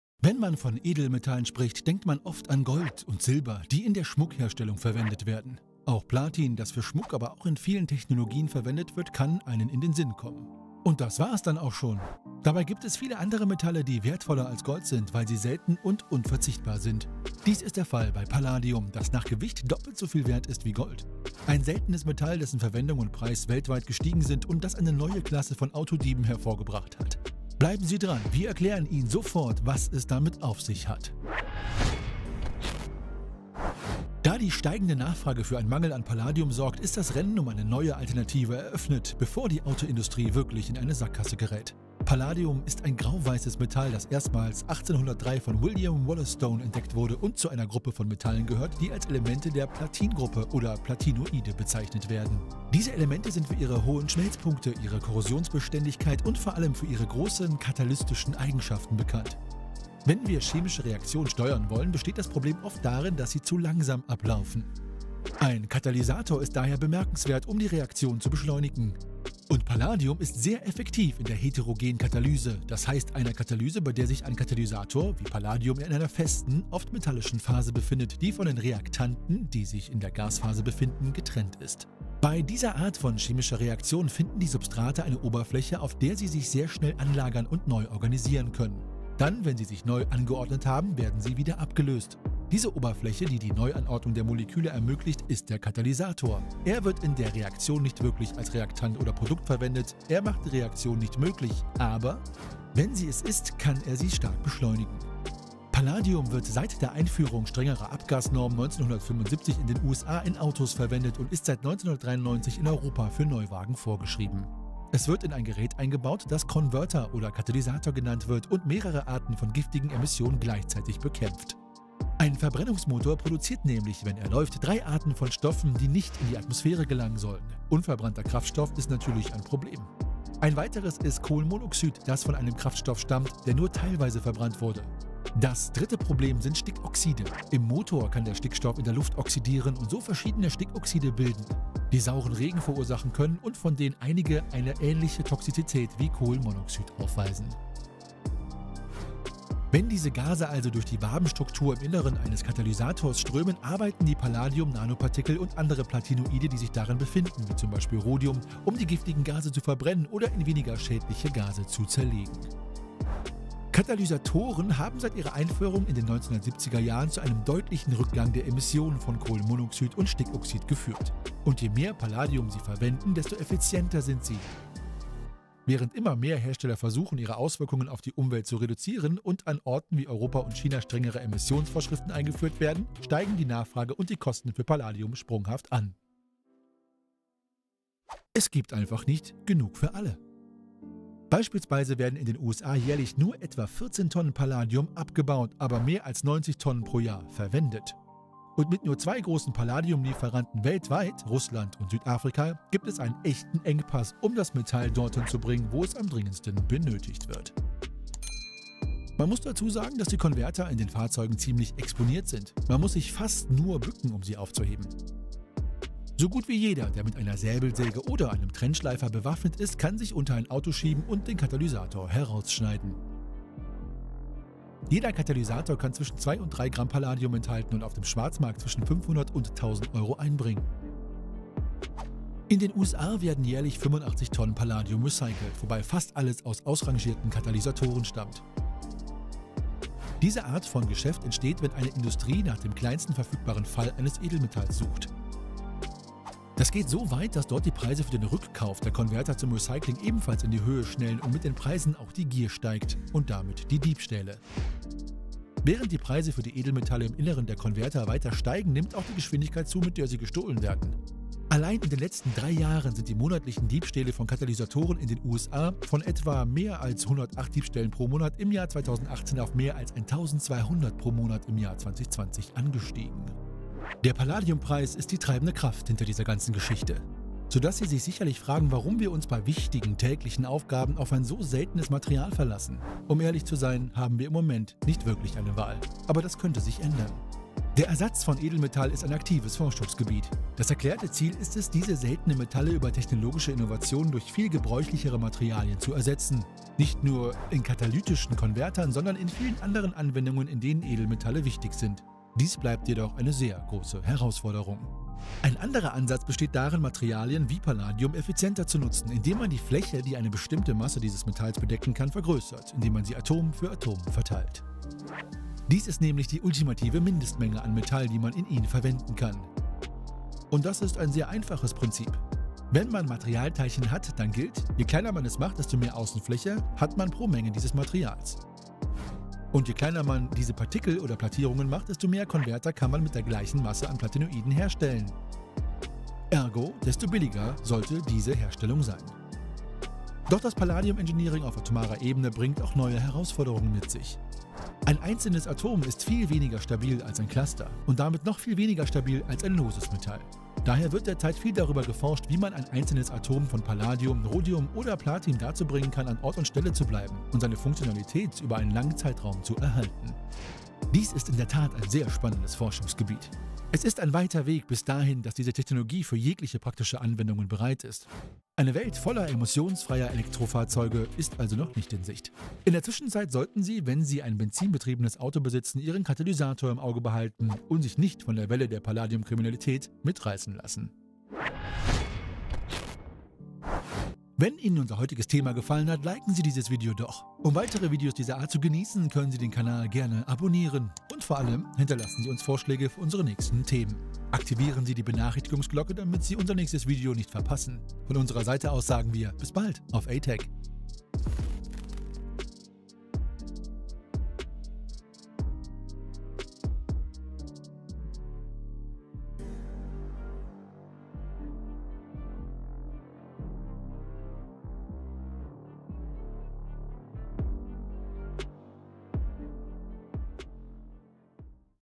Dieses Material ist sehr teuer, aber völlig unbekannt!
Wenn man von Edelmetallen spricht, denkt man oft an Gold und Silber, die in der Schmuckherstellung verwendet werden. Auch Platin, das für Schmuck, aber auch in vielen Technologien verwendet wird, kann einem in den Sinn kommen und dann ... Und das war es dann oft auch schon! Dabei gibt es viele andere Metalle, die wertvoller als Gold sind, weil sie selten und unverzichtbar sind. Dies ist der Fall bei Palladium, das gewichtsmäßig doppelt so viel wert ist wie Gold. Ein seltenes Metall, dessen Verwendung und Preise weltweit gestiegen sind und das eine neue Klasse von Autodieben hervorgebracht hat.

✔ Vielen Dank an Euch ヅ ヅ